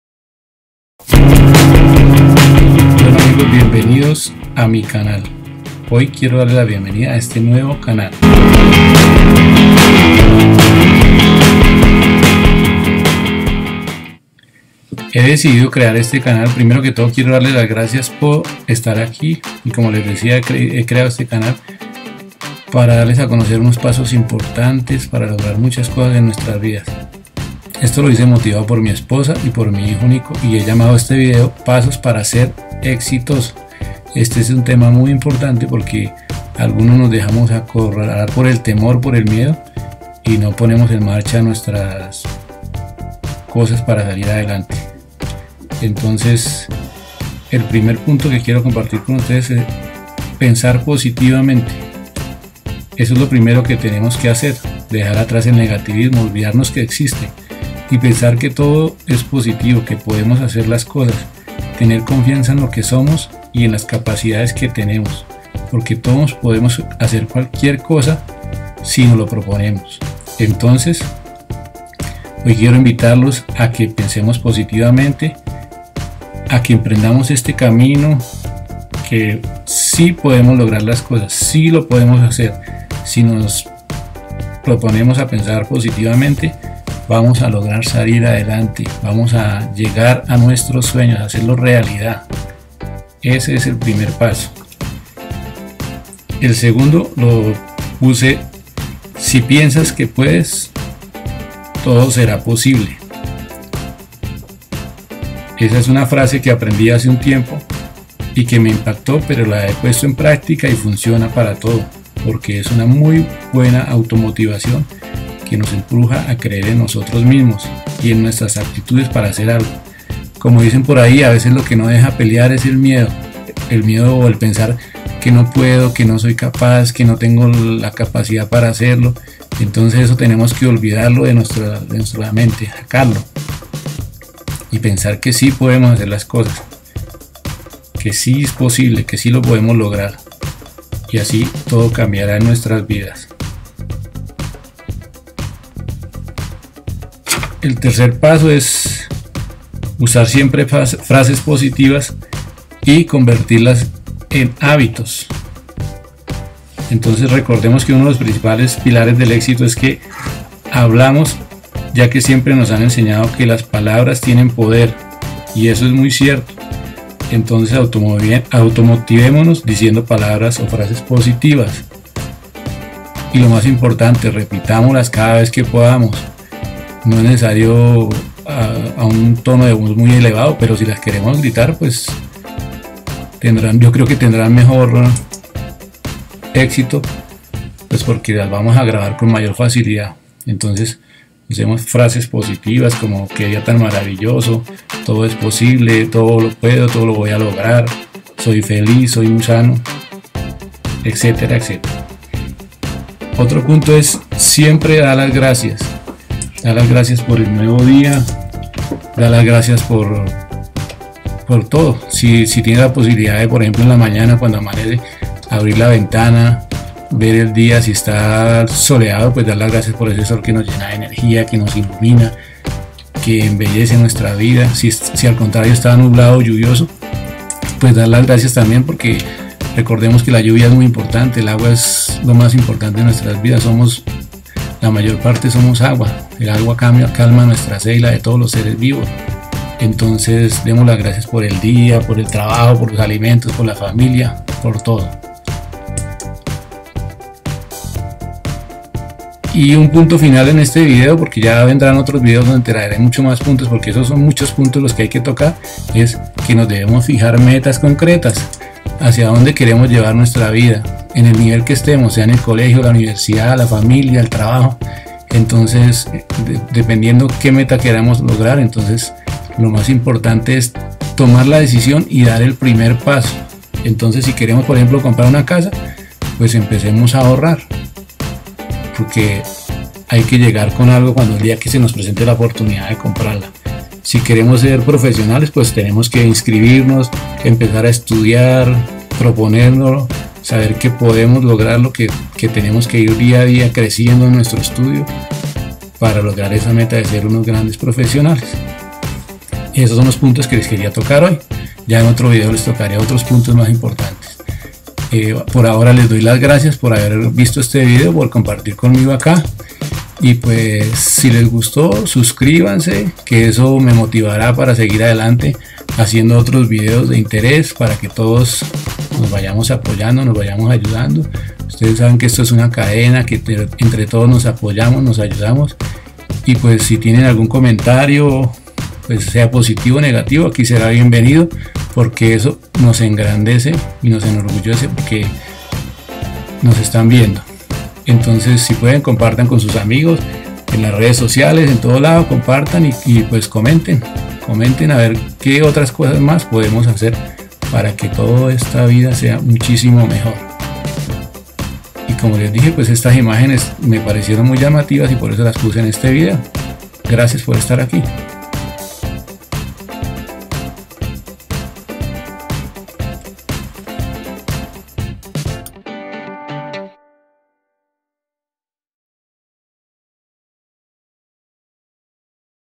COMO ALCANZAR EL EXITO pasos importantes
goo.gl/ZvTh1Q

EN ESTE VIDEO ENCONTRARA ALGUNOS PASOS IMPORTANTES QUE PONIENDOLOS EN PRACTICA PODRAS ALCANZAR EL EXITO Y MEJORAR EN TODO.PROGRAMA TU MENTE.